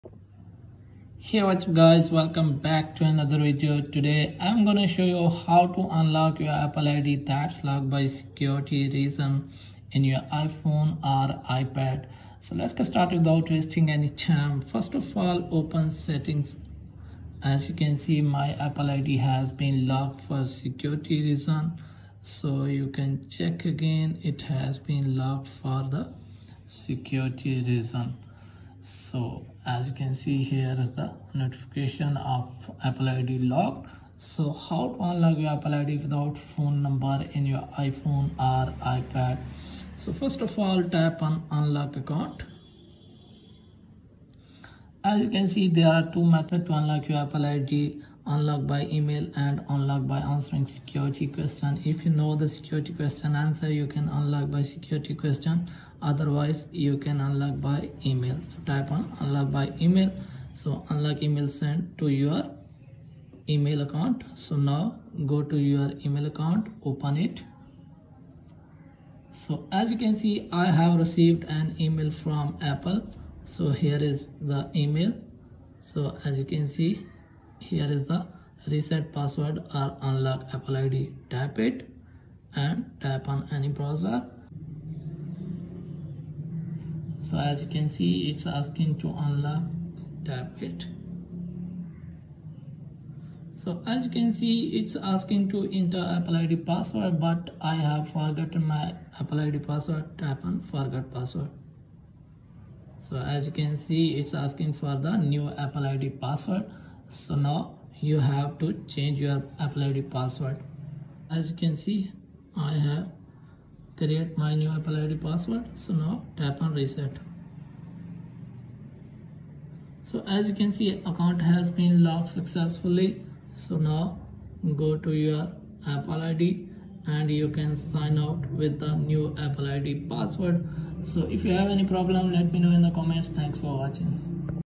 How to unlock apple id without phone number | Unlock Apple ID account without phone number | 2023
How to Unlock Apple ID account on iPhone without phone number iPhone iPad